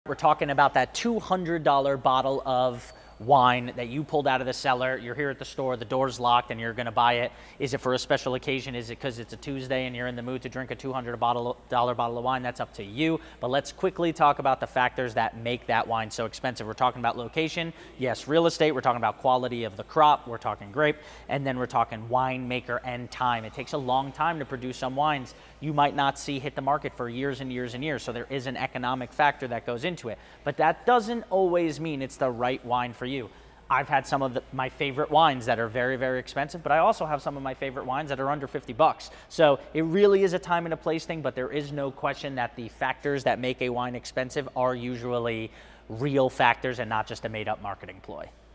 Is $200 wine worth it?! 😅
The simple answer is yes. Well, it being “worth it” at the end of the day is REALLY up to you because everyone has different ranges they shop in!

What we are talking about is, is it all hype or is there an actual reason that bottle of wine is upwards of 200 bucks!? When you take in all the factors of what goes into a bottle that expensive it really is “worth it” from a strictly dollars and cents standpoint!

You are getting a really high quality, hands on bottle that will really blow you away.

So if you are ever wondering if it’ll be worth it to break the bank for that special occasion and you appreciate the elements of what makes that bottle so expensive, it very well may be worth it!